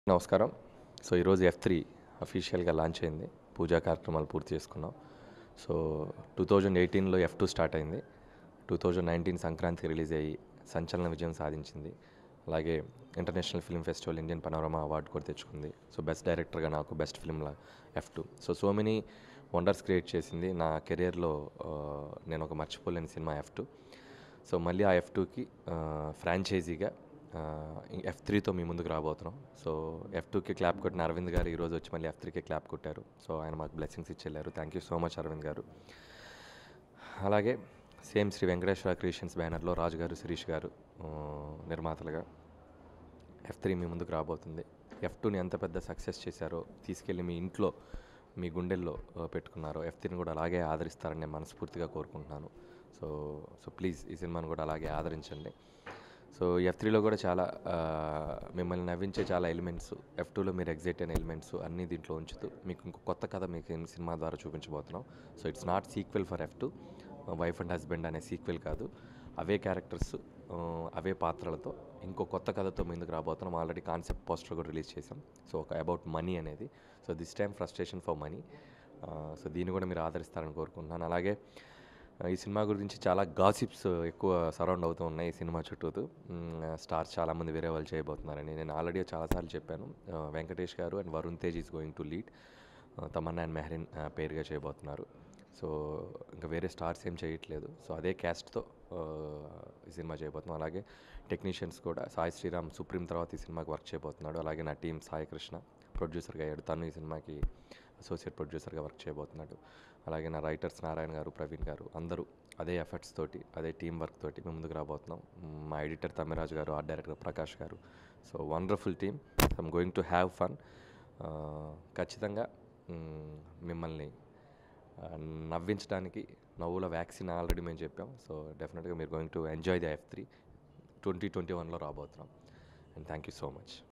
Director Anil Ravipudi about F3 Movie Story Line And Casting | F3Movie Pooja Ceremony | FIlmylooks
Watch Director Anil Ravipudi about F3 Movie Story Line And Casting | F3Movie Pooja ceremony | FIlmylooks